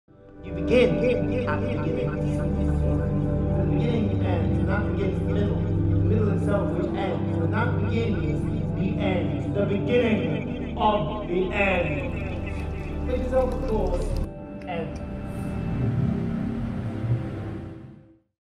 Tentacular Spectacular Trailer
13 - 17 June

Curated by the world’s premier, green, autistic drag queen Oozing Gloop, Tentacular Spectacular is an (ad)venture into networking networks of narrative, phobia, hope and utopia by showcasing the self made cryptids of 21st century trans* performers.

Oozing Gloop is joined by the architect of your eternal suffering, Olympia Bukkakis. Together, these Jesters of the new Dark Age will lure you through a psycho-sexual swamp of fester, rot & future relics. Emerging from this decay are the fruitful offshoots of this tentacular undergrowth; Shrek666, Bonnie Bakeneko and a mystery guest artist working with themes of monstrosity & mulch.